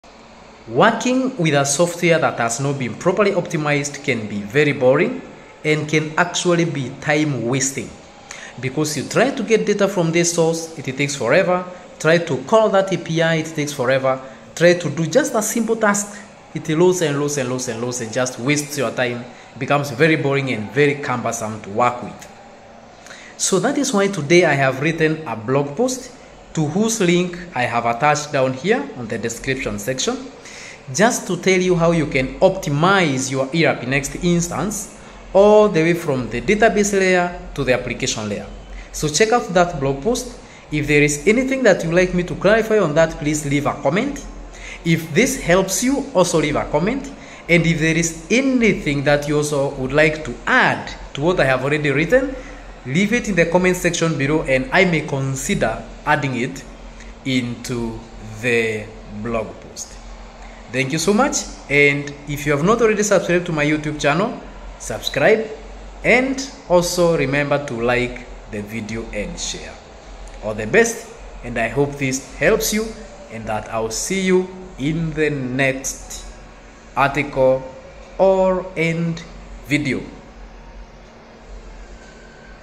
Optimizing ERPNext Performance: Tips for Faster and Smoother Operations
In this video, we introduce our latest blog post, "Optimizing ERPNext Performance: Tips for Faster and Smoother Operations." Discover essential strategies to enhance your ERPNext system's performance and ensure seamless operations.

    • Hardware and infrastructure optimization
    • Database indexing and maintenance
    • Application-level caching and background jobs
    • Writing efficient SQL queries
    • Client-side optimizations like minifying assets and lazy loading
    • Monitoring performance metrics and analyzing logs

Read the full article to unlock the full potential of your ERPNext system and boost your business efficiency.